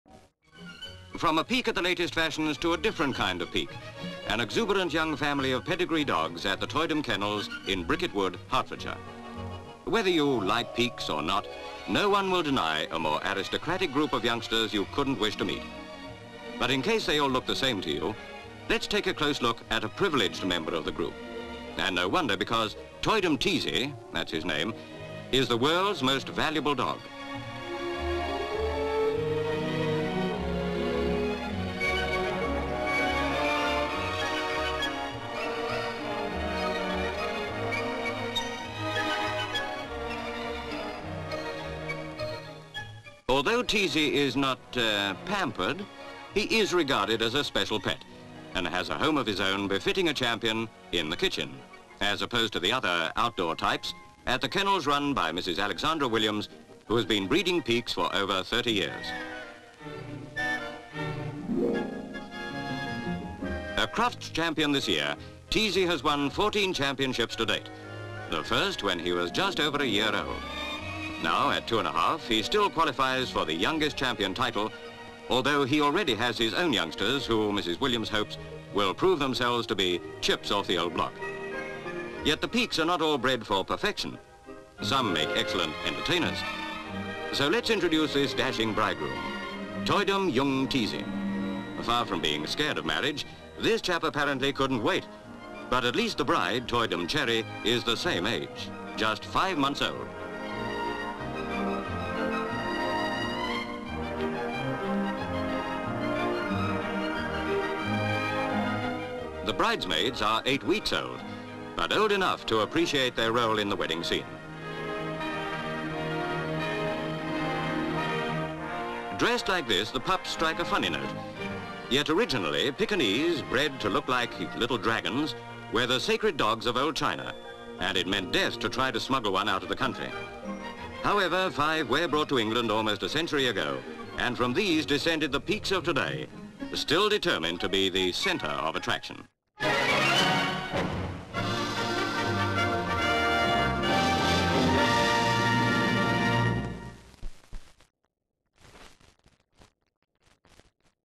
Champion Pekinese (1956)
Brickett Wood, Hertfordshire.

C/U of a group of Pekinese dogs in a garden of the Toydon Kennels.  M/S of the dogs milling about; a woman, Mrs Hurlock is seen petting them; Alexandra Williams, who runs the kennels, enters shot, picks up one of the dogs and carries him off.

Fluffing up the face fur of Toydom Ts'Zee (the dog), Alexandra takes him into the house and puts him down on a little grooming table by the fireplace. The Peke is then brushed, perfumed and powdered; two other Pekes stand on a dog basket outside the window and peek in (sorry) at the pampered pooch.  C/U of two rather soppy looking Pekes in separate dog baskets in the room.

C/U of Ts'Zee being brushed as commentator tells us he is a Crufts champion of this year and has won 14 championships to date; M/S of several trophies and a large shield on the mantelpiece.  C/U of a basket of five Pekinese puppies - Ts'Zee's offspring!

There then follows a gruesome sequence where Ts'Zee and his 'girlfriend', Toydom Cherrey, are seen in their wedding outfits!  He wears a suit and top hat, she wears a wedding dress, veil and earrings.  Even worse, two of the pups are dressed as bridesmaids with little bouquets (their paws must be tied to the flowers).  Alexandra and Mrs Hurlock hold the dogs in position as if for a wedding ceremony.  Quite unpleasant, though some may find it cute.

Note: on file are notes about the dogs and Mrs Williams, plus a news cutting from Reveille about Ts'Zee and his pampered life, and another article about another Peke, possibly considered for a story.
 FILM ID:41.12